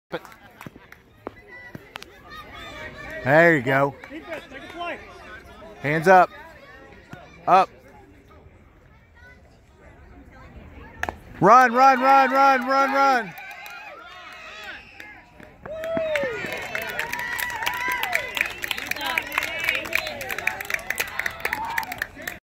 Bears v Rams, 2019.4.10, 2nd AB
Good swing, picked up her first of two RBIs today. Good effort in a win.